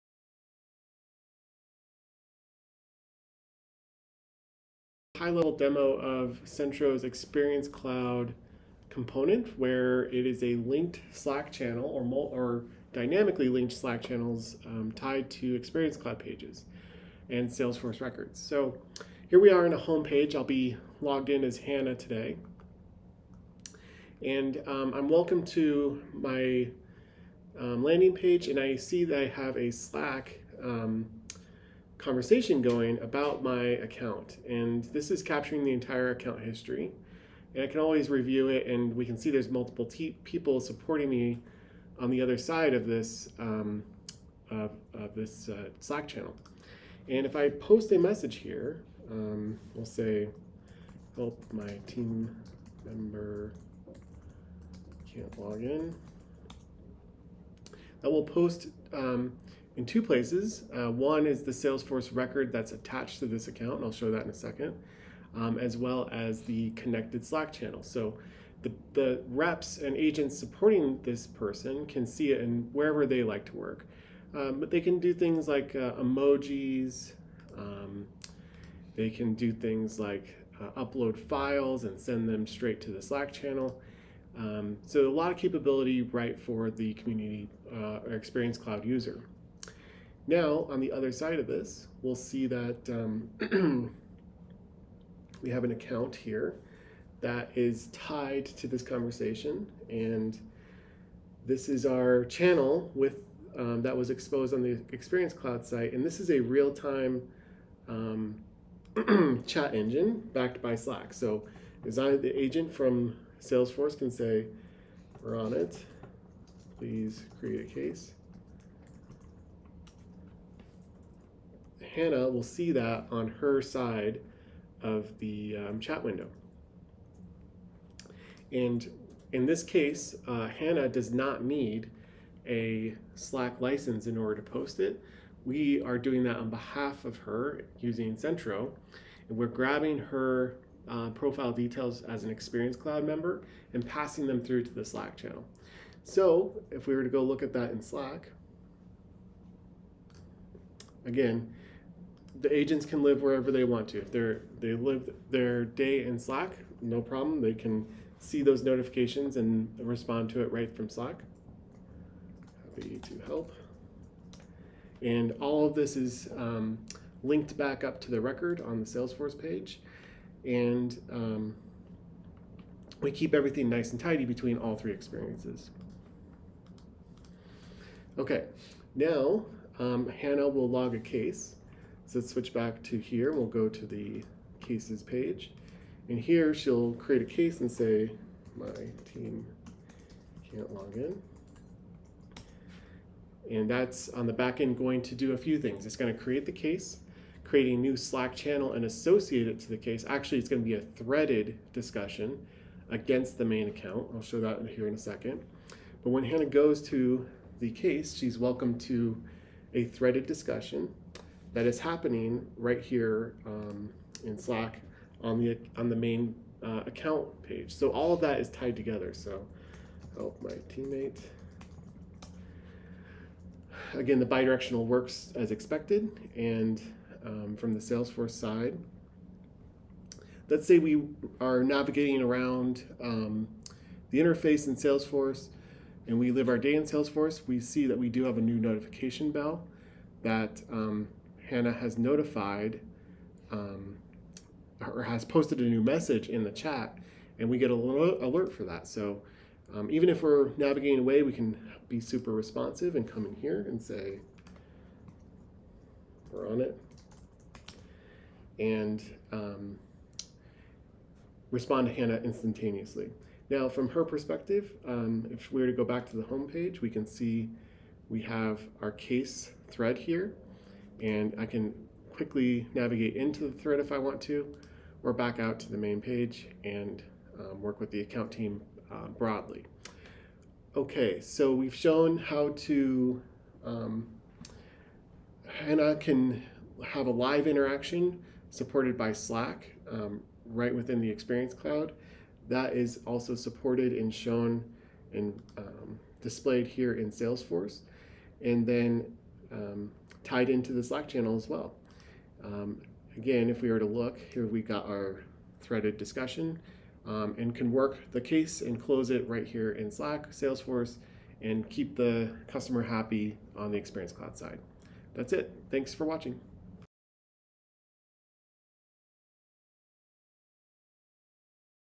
Embedded Slack In Experience Cloud - Explainer
Slack is the most responsive chat platform on the planet. Salesforce Experience Cloud sites are the most intuitive way to expose Salesforce data to Customers and Partners. Live chat on an experience cloud page is the perfect way for your reps to chat directly with customers! What better way to chat than Slack? Centro makes this possible with our embedded Slack widget for Experience Cloud pages and Salesforce Record. Check out this brief demo of this in action!